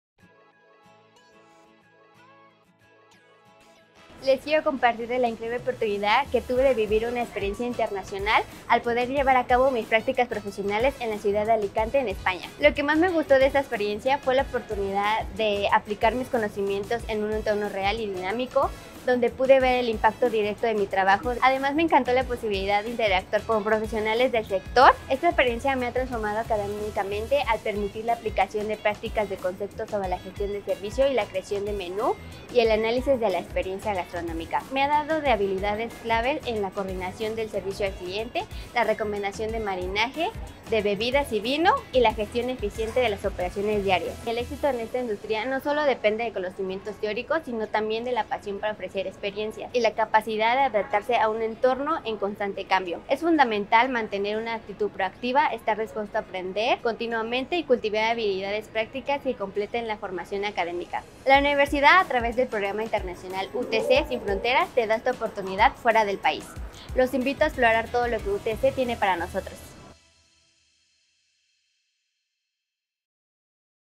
¡Descubre la experiencia de las prácticas profesionales de Lorena, egresada de Turismo UTC! ✈️🎓
🌍✨ Vive tus prácticas en el extranjero con UTC 🎓✈️

Amplía tus horizontes, conecta con otras culturas y fortalece tu CV con experiencias internacionales.